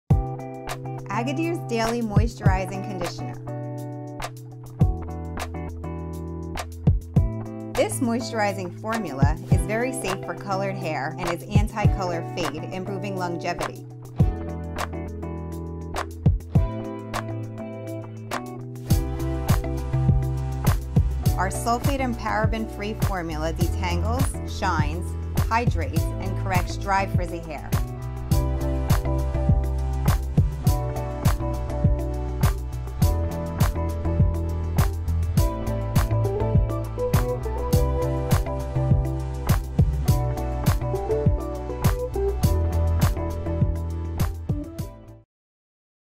Увлажняющий кондиционер Agadir
Увлажняющая формула абсолютно безопасна для окрашенных волос, предотвращает потерю насыщенности цвета волос, тем самым пролонгируя идеальный цвет. Подходит для ежедневного применения. Формула состава без сульфатов и парабенов. Способствует распутыванию волос. Насыщает блеском
и сиянием, увлажняет и восстанавливает сухие, вьющиеся волосы.